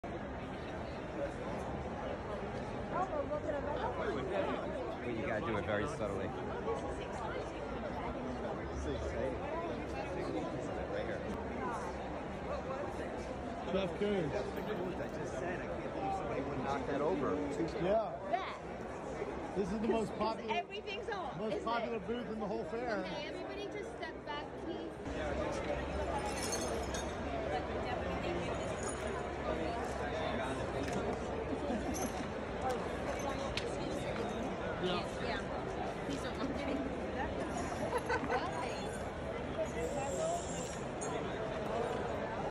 Jeff Koons Sculpture Shatters Into Pieces At Art Wynwood Miami, Audience Stunned | Florida
Visitors to an art gallery stand around as pieces of a damaged Jeff Koons sculpture lie on the floor at Art Wynwood in Miami, Florida. Video credit: Stephen Gamson/@gamsonart